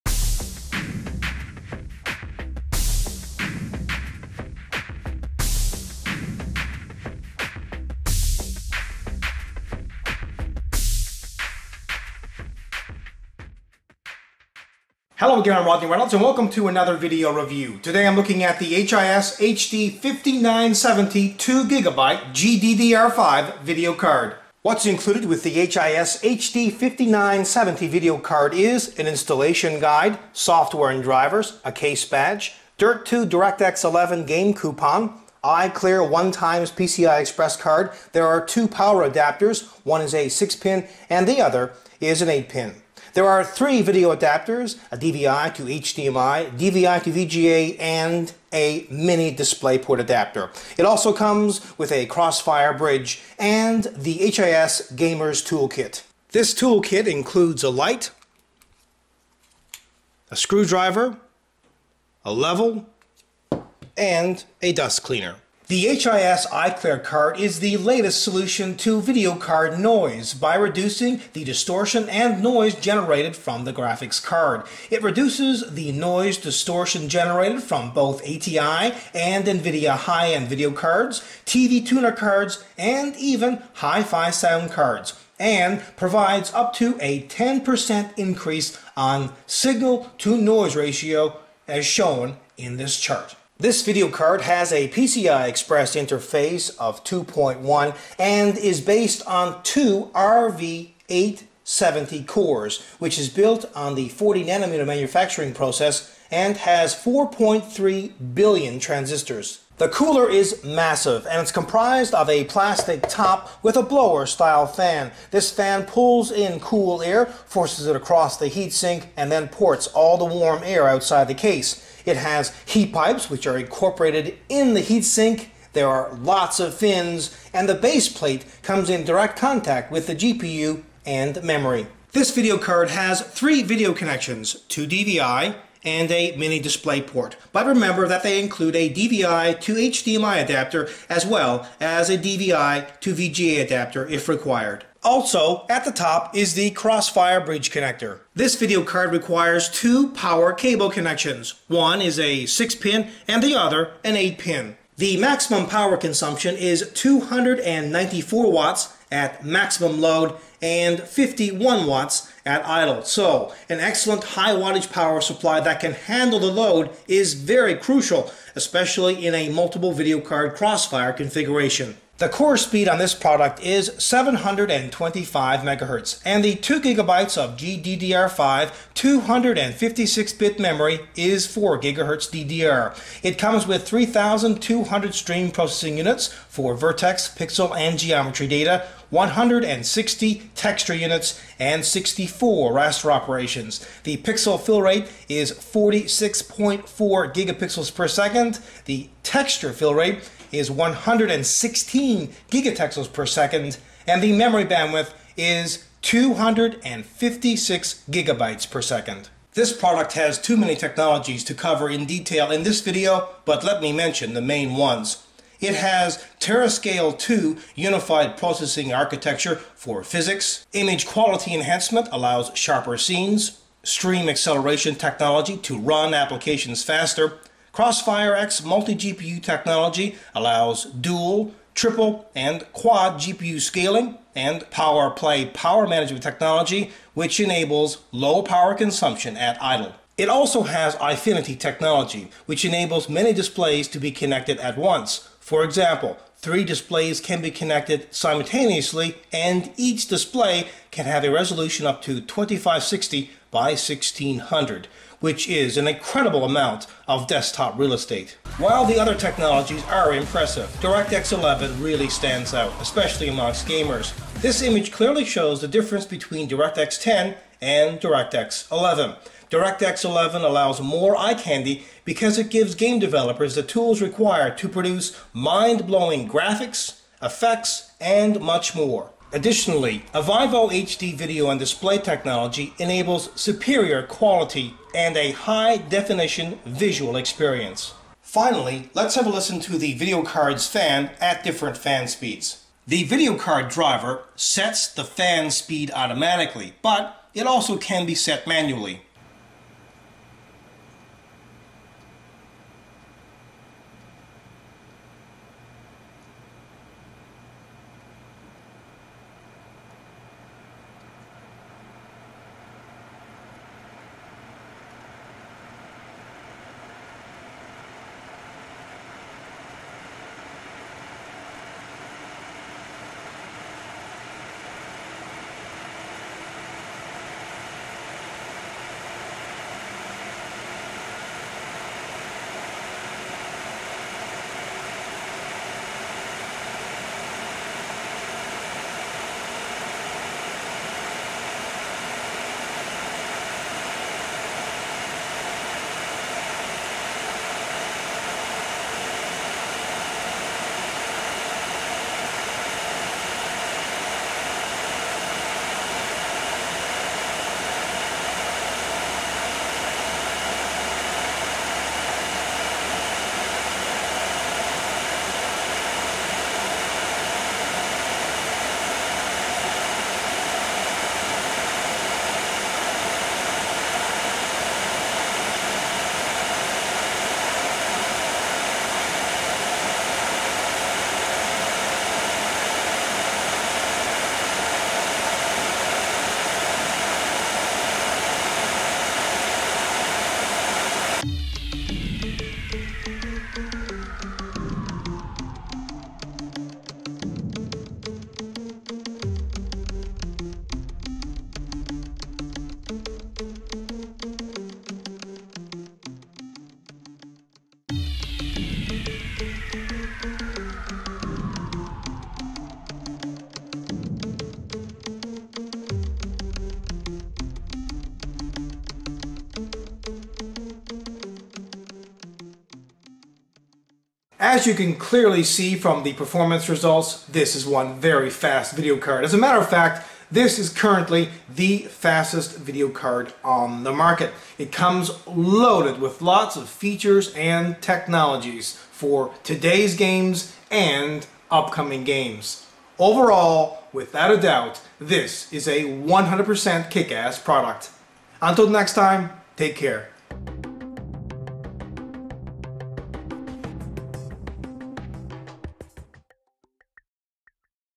#1114 - HIS HD 5970 2GB GDDR5 Video Card Video Review
The HIS HD 5970 2GB GDDR5 Video Card has a core speed on this product is 725MHz and the 2GB of GDDR5 256 bit memory is 4GHz DDR. It comes with 3200 stream processing units for vertex, pixel and geometry data, 160 texture units and 64 raster operators. The pixel fillrate is 46.4 Gpixels/sec, texture fillrate is 116 GTexels/s and the memory bandwidth is 256GB/s.